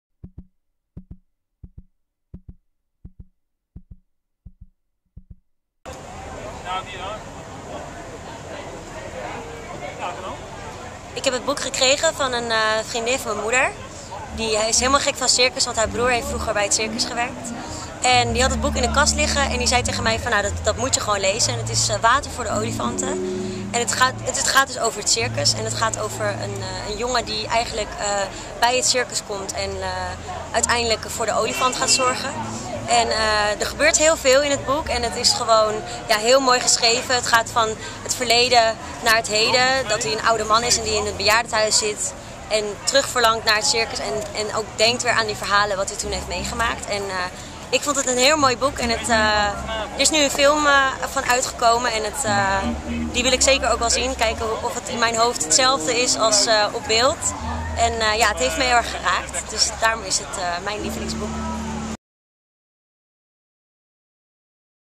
Barbara over Water voor de Olifanten - Sara Gruen
- BOEKENTIPS IN 1 MINUUT - "Ze zei: "Dat moet je gewoon lezen.""   "Ik vond het een heel mooi boek. Er is nu een film van uitgekomen. Die wil ik zeker ook zien."   "Het heeft me heel erg geraakt. Daarom is het mijn lievelingsboek."   Barbara is Danseres/Actrice, 23 jaar en woont in Beverwijk.